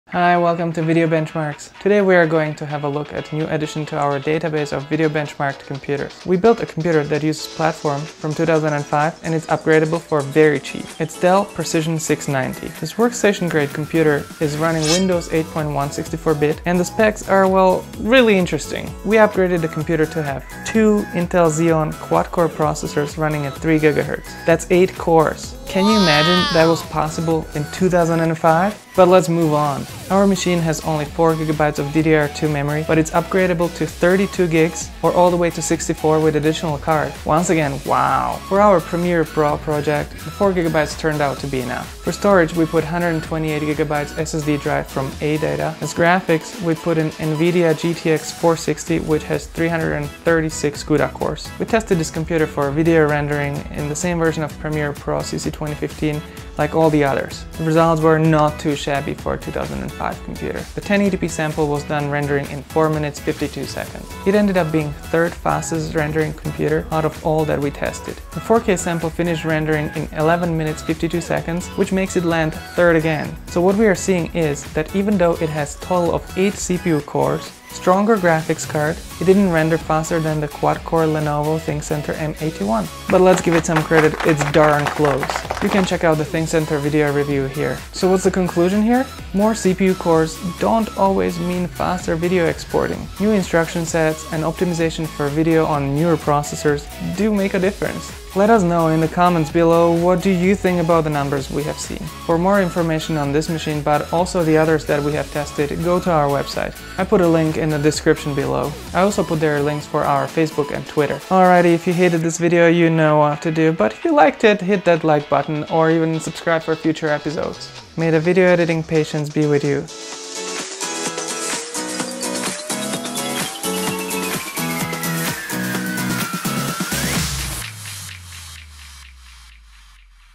Video Render Speed: 8 core beast from 2005
How long will your videos render in Adobe Premiere Pro CC 2015 on a Dell Precision 690?

Photos are courtesy of: hardware.info

Amazon Canada:

Amazon US: